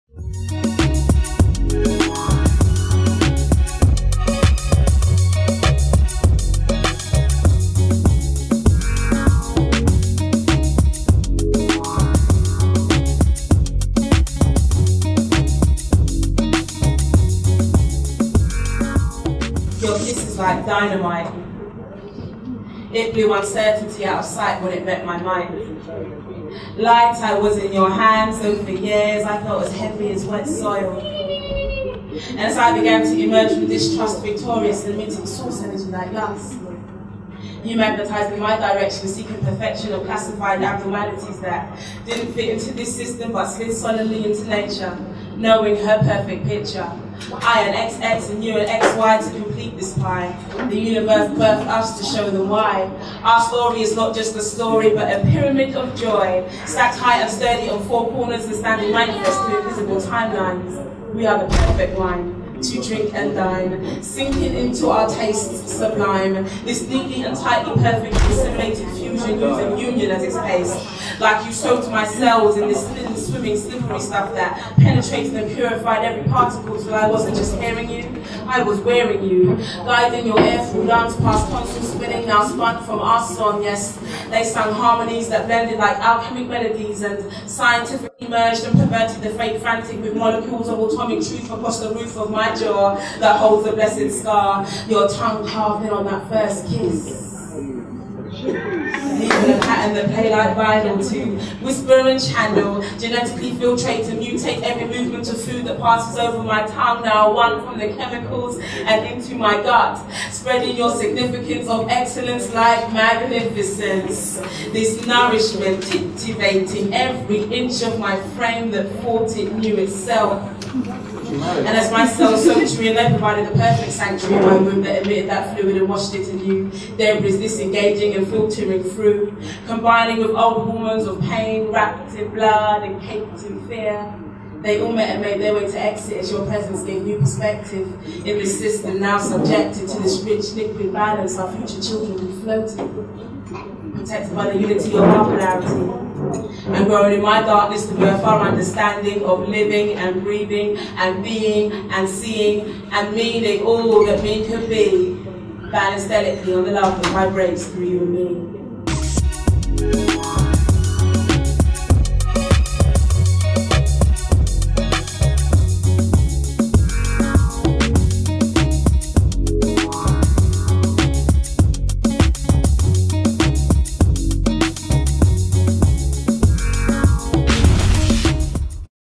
Lets Talk - London,UK ft. 2
Discussion/ Spoken word event that takes place every 2nd Monday -'Talk' in London, UK.